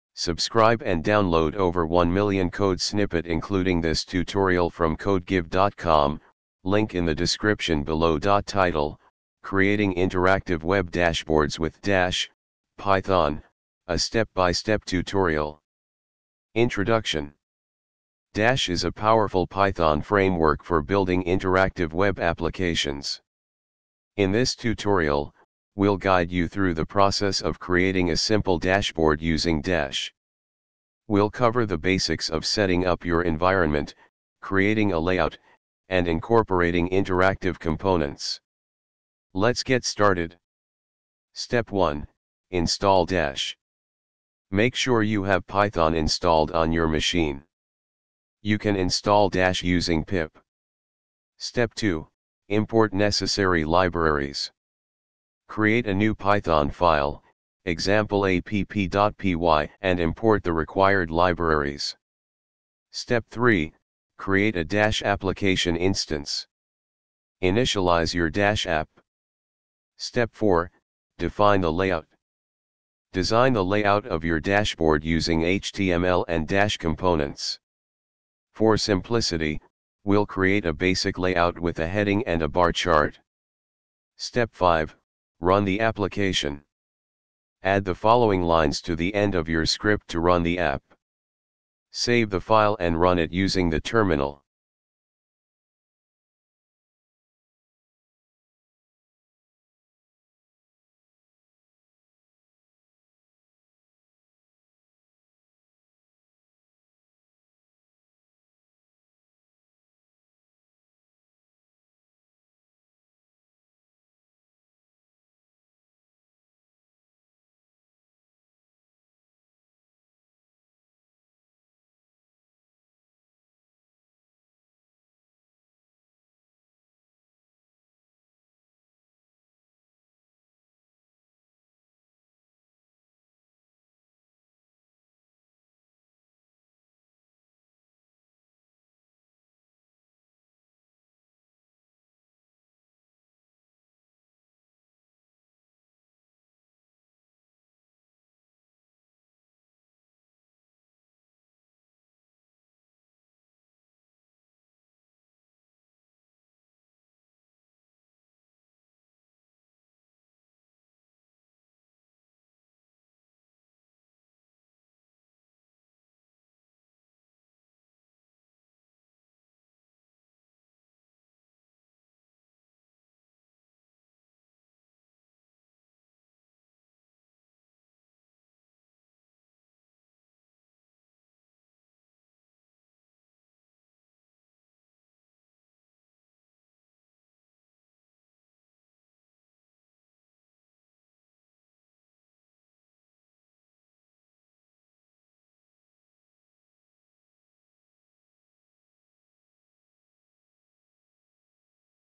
dash python logo
Title: Creating Interactive Web Dashboards with Dash (Python) - A Step-by-Step Tutorial
Introduction:
Dash is a powerful Python framework for building interactive web applications. In this tutorial, we'll guide you through the process of creating a simple dashboard using Dash. We'll cover the basics of setting up your environment, creating a layout, and incorporating interactive components. Let's get started!
Step 1: Install Dash
Make sure you have Python installed on your machine. You can install Dash using pip:
Step 2: Import necessary libraries
Create a new Python file (e.g., app.py) and import the required libraries:
Step 3: Create a Dash application instance
Initialize your Dash app:
Step 4: Define the layout
Design the layout of your dashboard using HTML and Dash components. For simplicity, we'll create a basic layout with a heading and a bar chart:
Step 5: Run the application
Add the following lines to the end of your script to run the app:
Save the file and run it using the terminal: